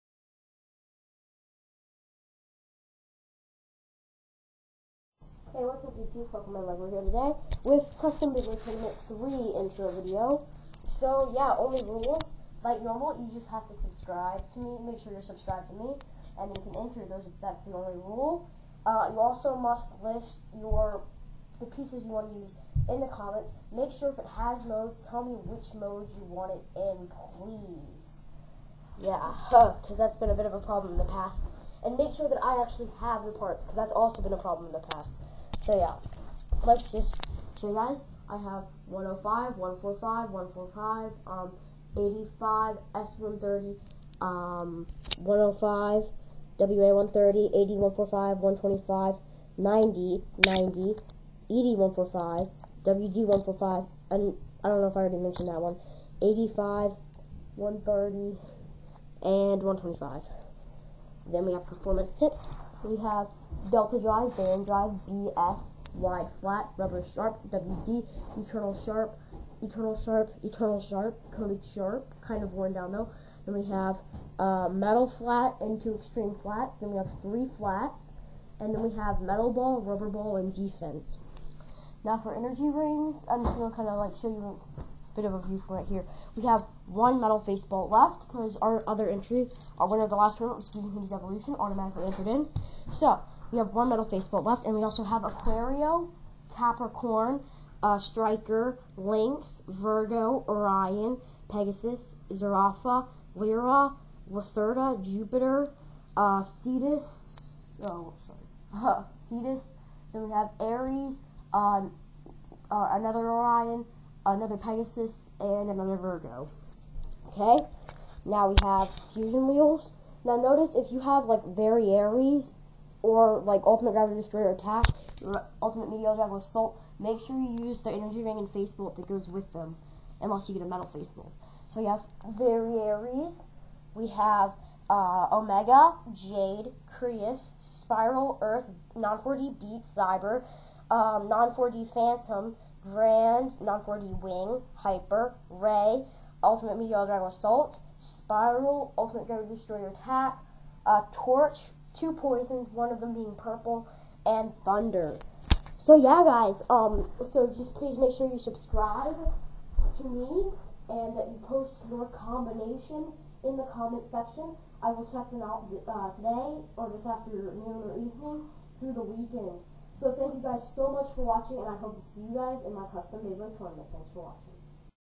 Custom Beyblade Tournament 3 Intro Video.wmv (Closed)
Custom Beyblade Tournament 3 Intro Video.wmv Rules to enter: Be subscribed to my channel, enter valid beyblade parts in the comment section (Tell me what mode you want them in) Have fun!
1. FusionHadesEvolution MF Phantom (Stamina mode) Cygnus GB145WD
2. Showerman2k11 MF VariAres 105MF (Right Spin)
3. Mattsbeybattles Kreis (Defense Mode) Aquario AD145WD
4. GilaSpiderLizard Spiral Pegasus S130WF
5. ALIXXBB Jade Orion BD
6. James Banks Poison Virgo 105ES
7. Darkest Blader Earth Jupiter 145D
8. BladerNINJA09 Omega (Assault mode) Capricorn DD (WB mode)
9. IHaveTheBeySpirit Spiral Virgo 130XF
10.gabrielrendzbovideo Gravity Destroyer Att. WA130XF (Right Launch)
11. flo2777 Grand Striker ED145MB
12. masumune kadoya Ray Orion D125RB
13. ExoticsKing Ult. Meteo L-Drago Assault WD145BS
14. Isiah Thomas Non 4D Phantom Pegasus 85CS
15. jay henderson Non 4D Beat Zurafa 145RS
16. CHARIZARDMASTER122dn Cyber Lyre 125F